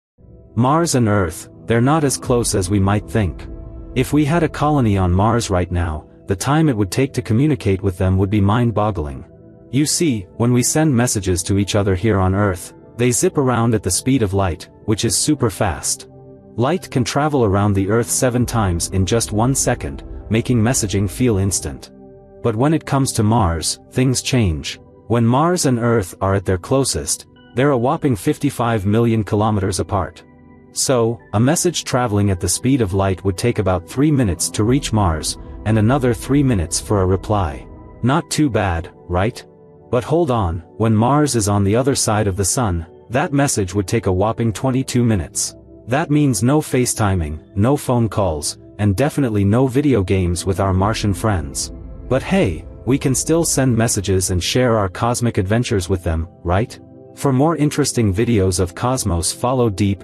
Communication between Mars & Earth, Colony on Mars
Mars and Earth, they're not as close as we might think. If we had a colony on Mars right now, the time it would take to communicate with them would be mind-boggling. You see, when we send messages to each other here on Earth, they zip around at the speed of light, which is super fast. Light can travel around the Earth seven times in just one second, making messaging feel instant. But when it comes to Mars, things change. When Mars and Earth are at their closest, they're a whopping 55 million kilometers apart. So, a message traveling at the speed of light would take about three minutes to reach Mars, and another three minutes for a reply. Not too bad, right? But hold on, when Mars is on the other side of the Sun, that message would take a whopping 22 minutes! That means no Face Timing, no phone calls, and definitely no video games with our Martian friends. But hey, we can still send messages and share our cosmic adventures with them, right?